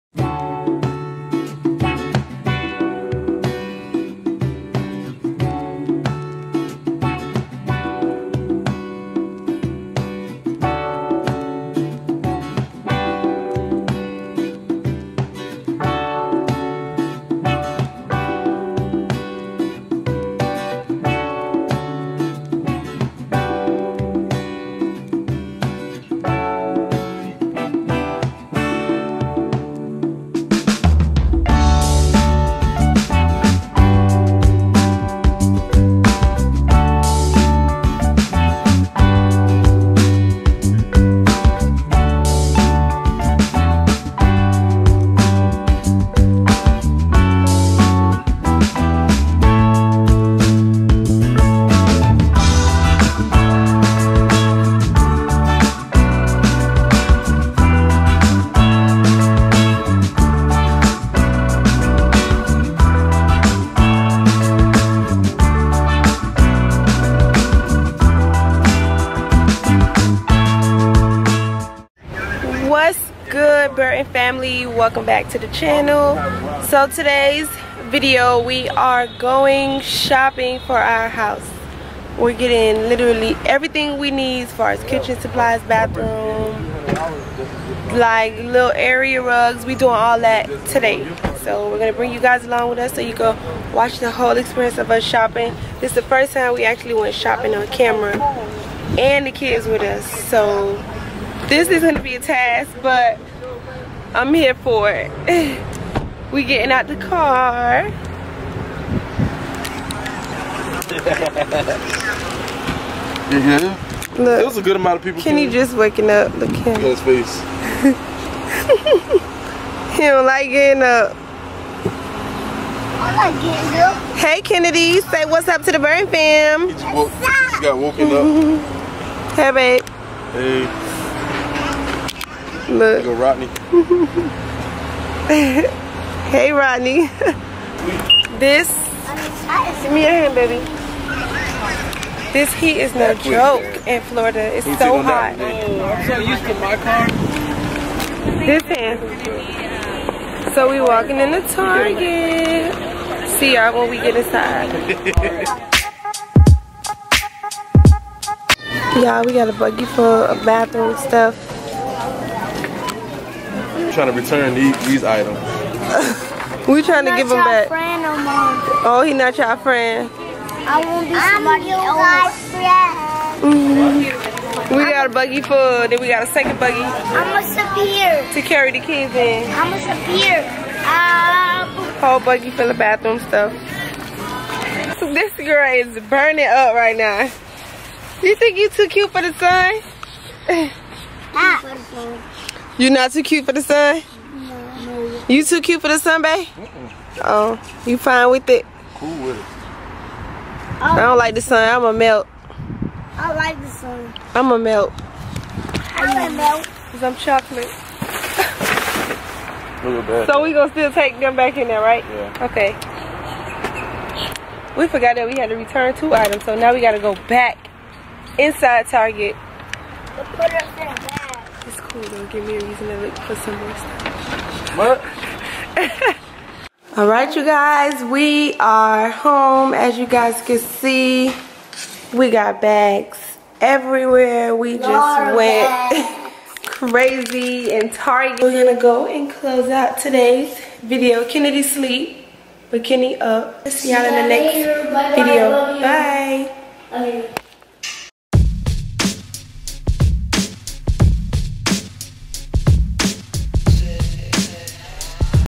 HOUSE SHOPPING VLOG | AT TARGET
For the first time we went shopping on camera and we brought our kids along to make it more fun.. We brought things for the kids bathroom, guest bathroom and kitchen supplies. Enjoy our fun shopping vlog!

Lori: @lillori
Kenny: @mnimnb
Family Ig: @burtonfamilyy